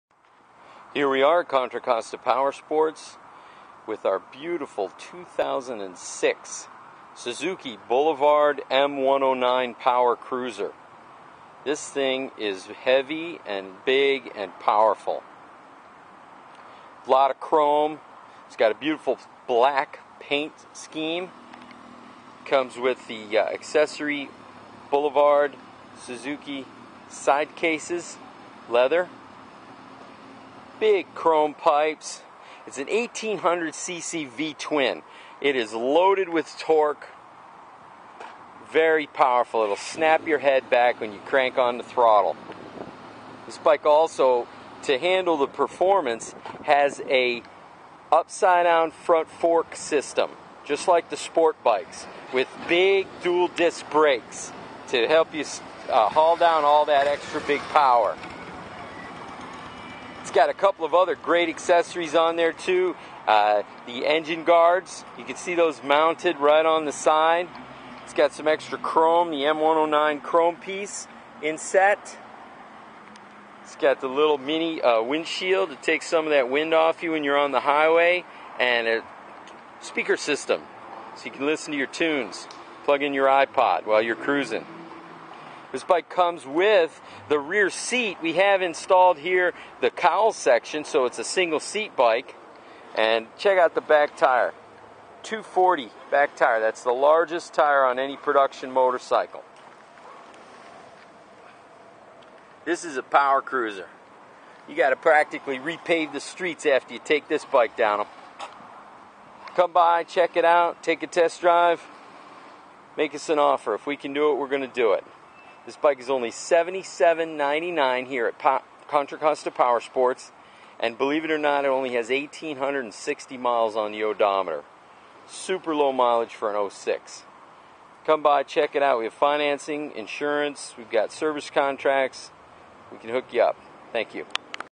Contra Costa Powersports-Used 2007 Suzuki M109R Boulevard Power Cruiser
We buy Used Clean Title motorcycles, come by for an appraisal!

This is a very clean, heavy, powerful, torquey, Boulevard/V-Rod Crusher and is for sale at Contra Costa Powersports for only $7,799. Financing is available along with a warranty. Trades are welcome, paid for or not.